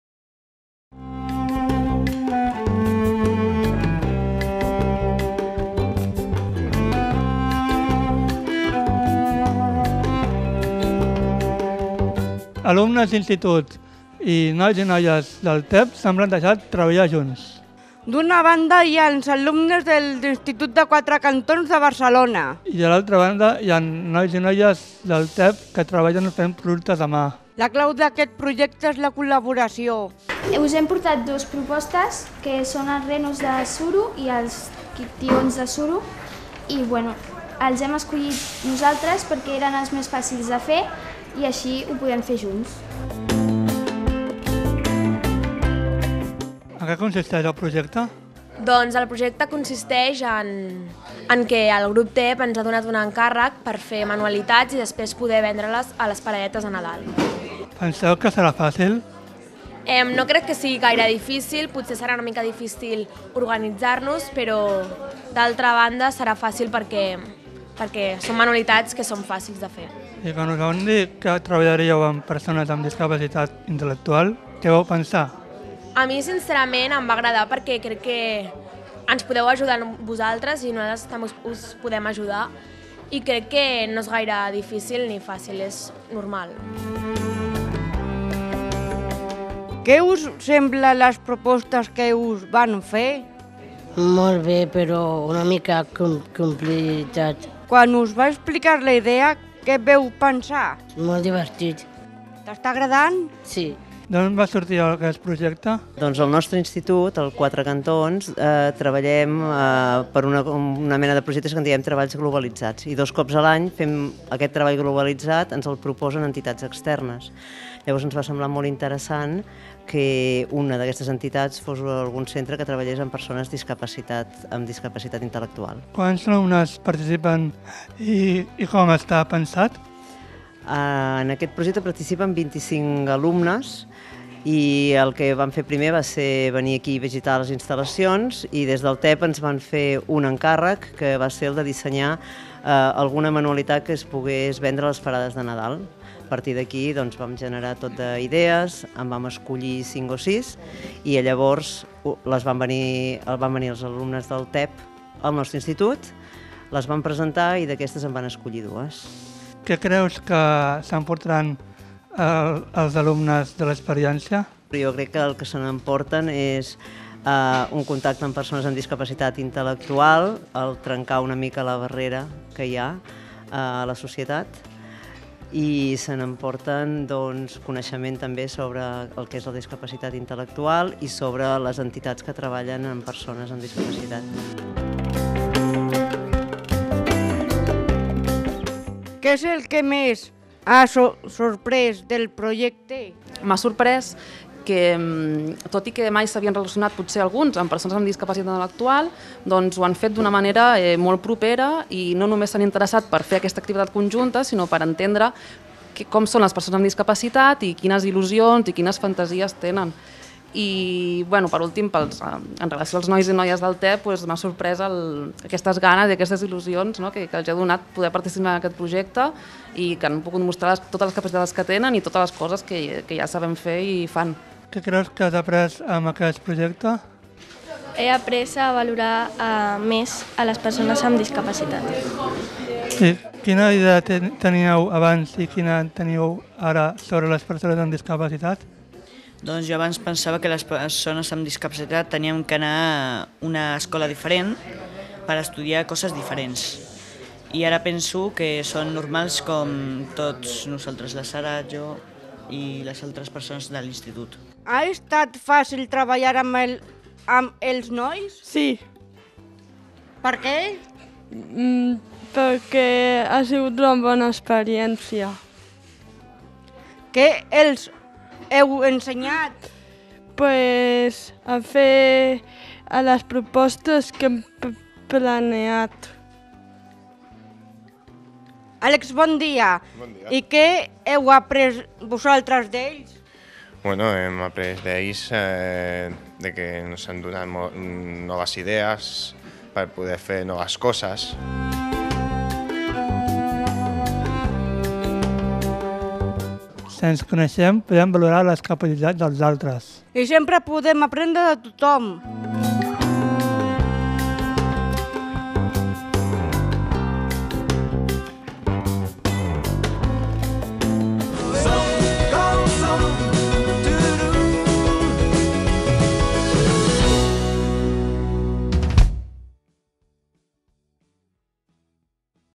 Capacitats per aprendre i per ensenyar
La millor manera de trencar barreres i vèncer els prejudicis sobre les persones amb discapacitat intel·lectual és conèixer la seva realitat i posar-se a la seva pell. Això és el que han pensat a l’institut Quatre Cantons de Barcelona. Alumnes de primer curs d’aquest centre de Secundària han treballat de forma cooperativa amb nois i noies del Servei Ocupacional d’Inserció del TEB Sant Andreu. L’objectiu era fer manualitats… però sobretot conèixer el talent dels altres i comprovar que tots estem capacitats per aprendre i per ensenyar.